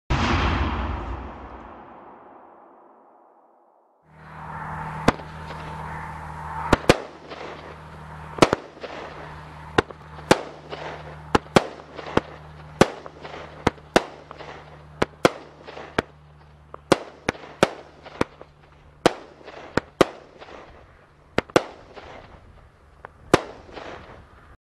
Money Box (World Class Fireworks)
Orange, skyblue dahlia and silver giltter/ sky blue, lemon dahlia and silver glitter/ dark red, skyblue dahlia and silver glitter.

13 Shots